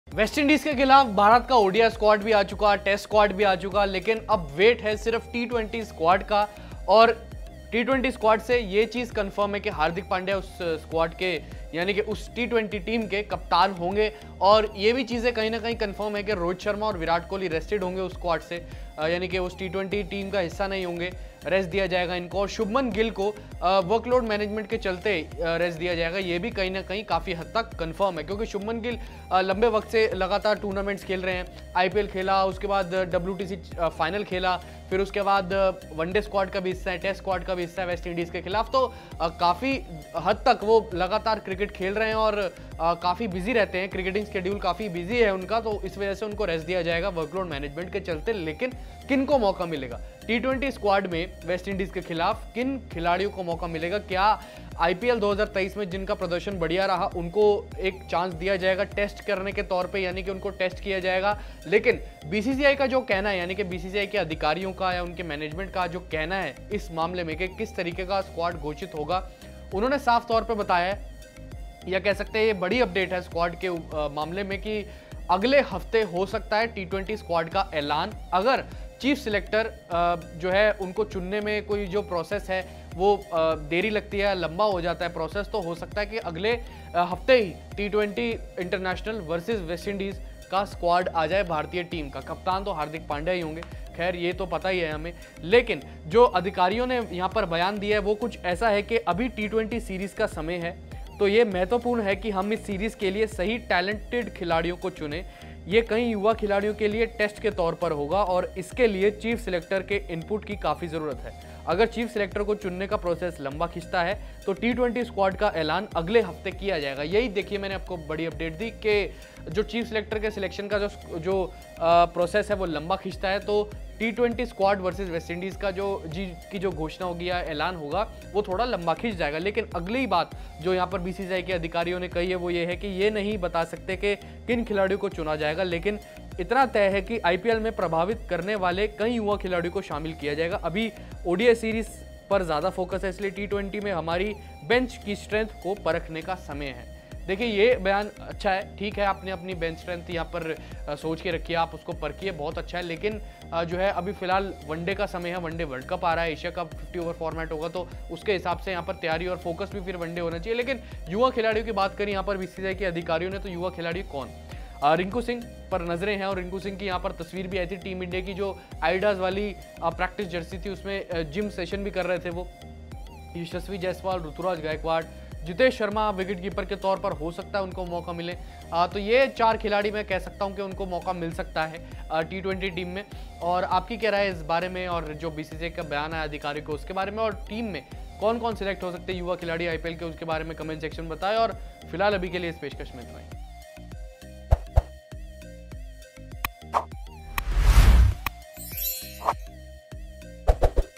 IND Vs WI: Hardik Pandya ने चुने अपने पसंदिदा चेहरे, इस दिन होगा WI के खिलाफ T20 I Squad का ऐलान
Hardik Pandya ने चुने अपने पसंदिदा चेहरे, इस दिन होगा WI के खिलाफ T20I Squad का ऐलान | IND Vs WI
Anchor: ritik kapoor
Editor: rajpal